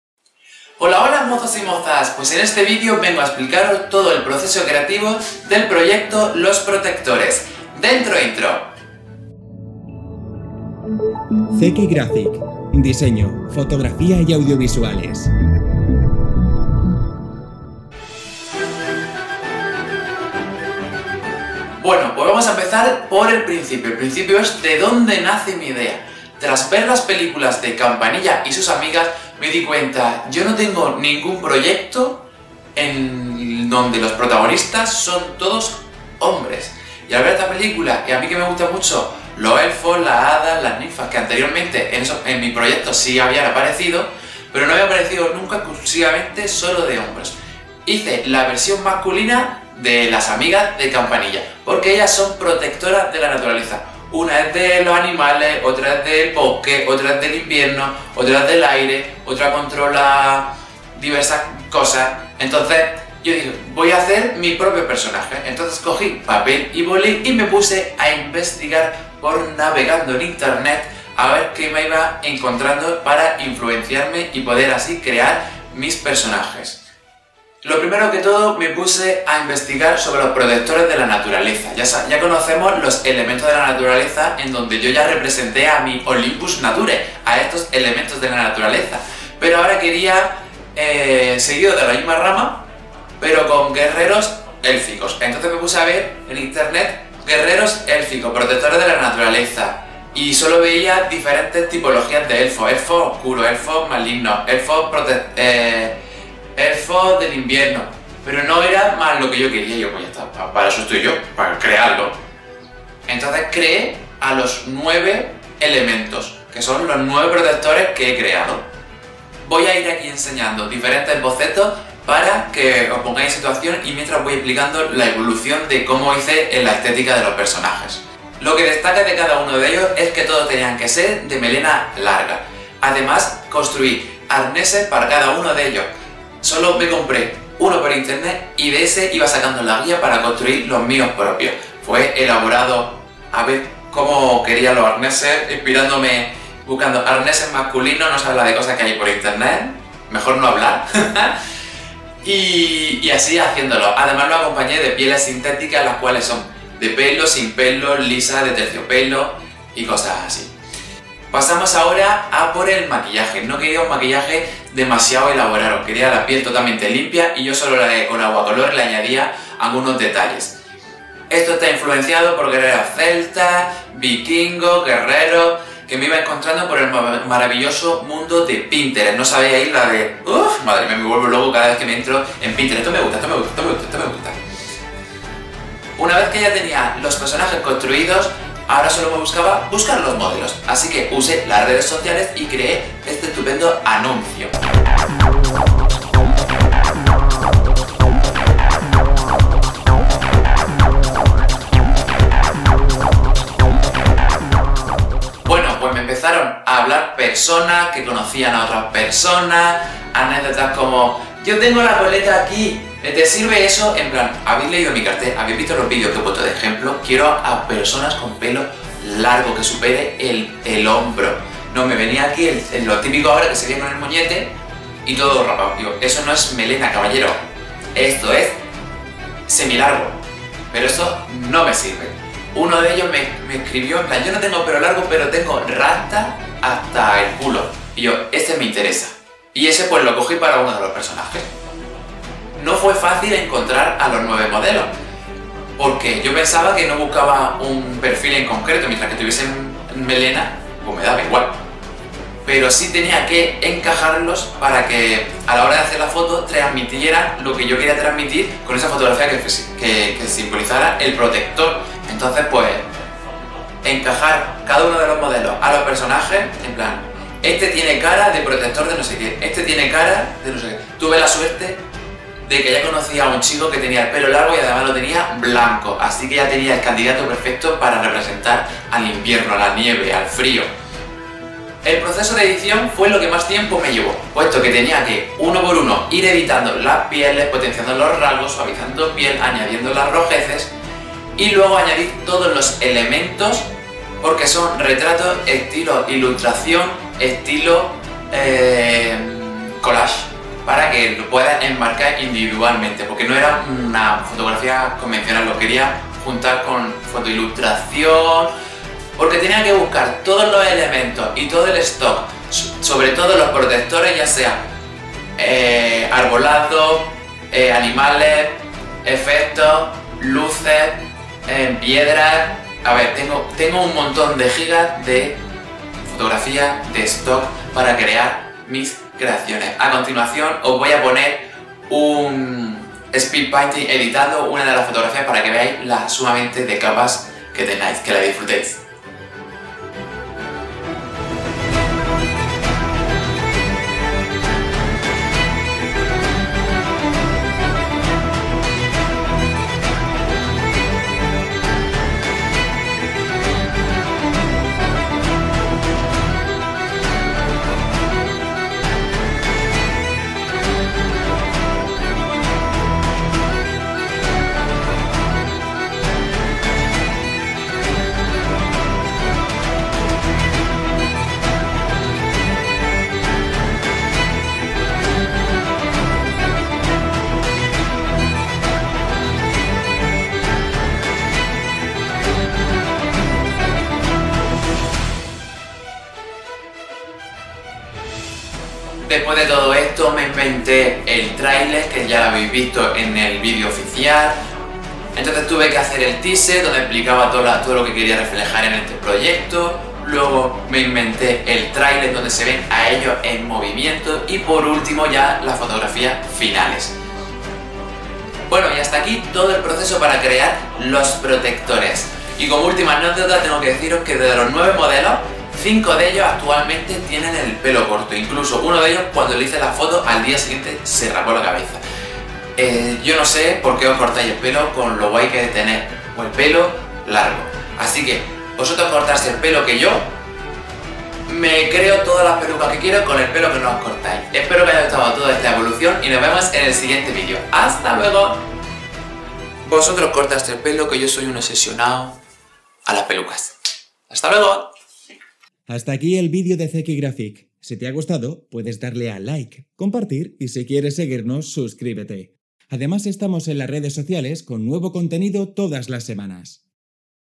Los Protectores | Proceso Creativo, Making Of
El proyecto visual, "Los Protectores" te enseño todo el proceso creativo, para seber mas visita ⬇⬇⬇Estamos en las Redes⬇⬇⬇